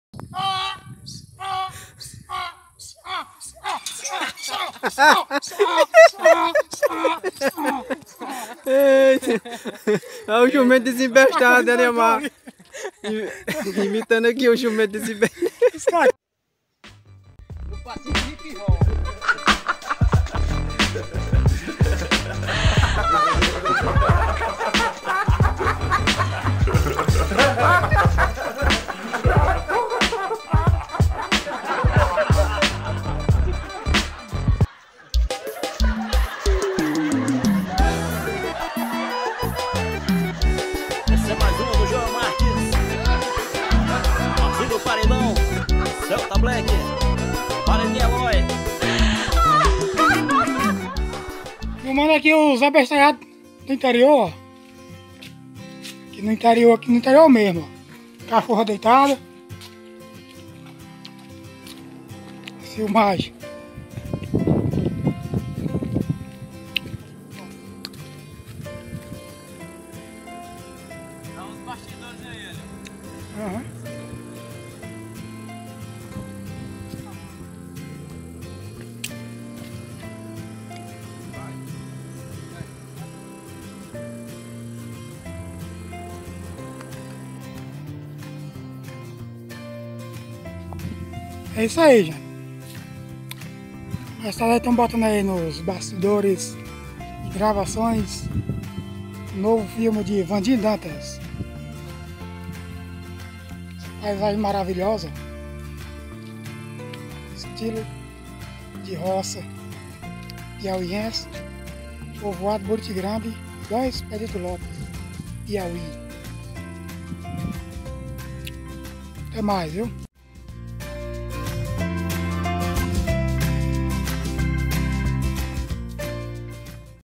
IMITANDO JUMENTO DESEMBESTADO - DANÇA DO ELIOMAR - MAKING OF
Eliomar Gonçalves aprontando nos bastidores do filme Abestaiados do Interior